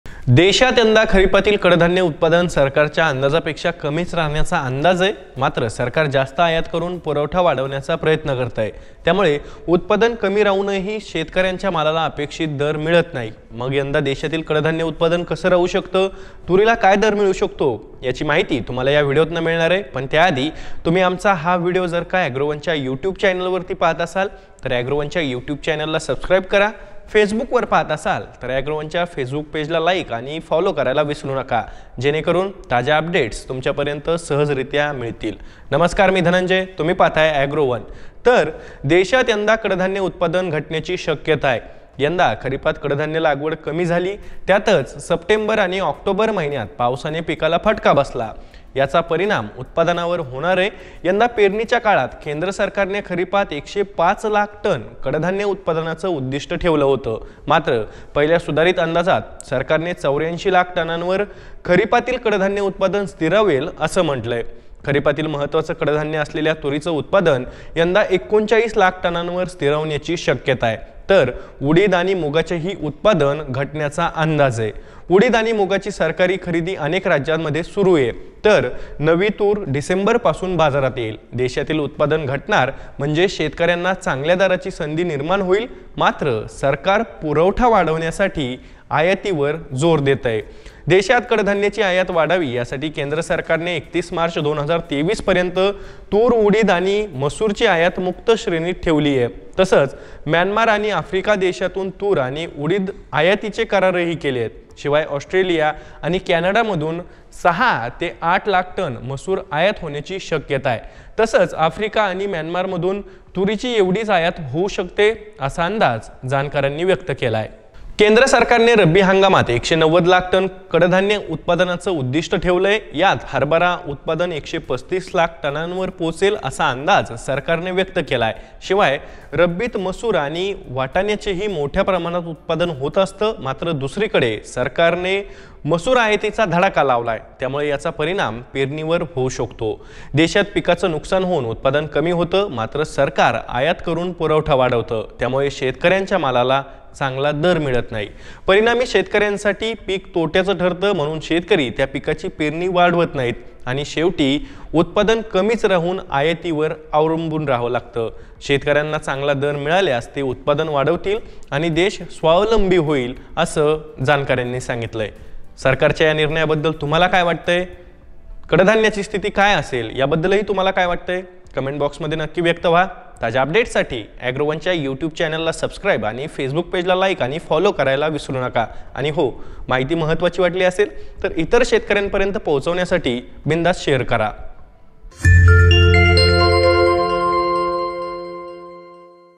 Pulses Rate : कडधान्य आयात वाढविण्याचा सरकारचा प्रयत्न | Agrowon | ॲग्रोवन
देशात यंदा खरिपातील कडधान्य उत्पादन सरकारच्या अंदाजापेक्षा कमीच राहण्याचा अंदाज आहे. मात्र सरकार जास्त आयात करून पुरवठा वाढवण्याचा प्रयत्न करतंय. त्यामुळं उत्पादन कमी राहूनही शेतकऱ्यांच्या मालाला अपेक्षित दर मिळत नाही. मग यंदा देशातील कडधान्य उत्पादन कसं राहू शकतं? तुरीला काय दर मिळू शकतो? याची माहिती तुम्हाला या व्हिडिओतून मिळेल.

This year's pulse production in the country is expected to be less than the government's estimate. But the government is trying to increase the supply by importing more. Due to this, despite the low production, the farmers' goods are not getting the expected price. So how can the pulse production in the country this year? What rate can Turi get? You will get this information from this video.

► Sakal Media Podcasts:

आमच्याशी जोडून राहण्यासाठी भेट द्या